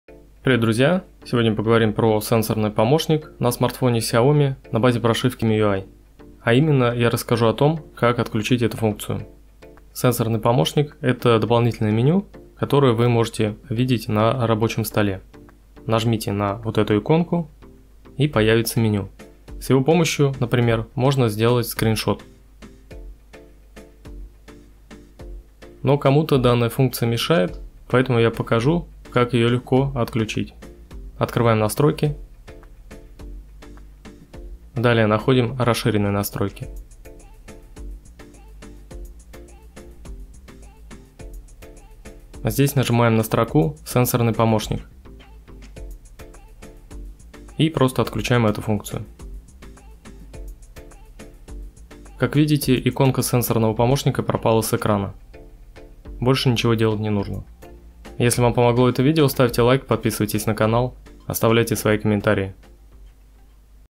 Как отключить сенсорный помощник на Xiaomi?
Если вы случайно включили сенсорный помощник на смартфоне Xiaomi на базе MIUI, его очень легко отключить - с помощью настроек. Показываю, как это сделать.

1. Идем в настройки.
2. Далее - "Расширенные настройки".
3. Тапаем по строке "Сенсорный помощник".
4. Отключаем функцию с помощью переключателя.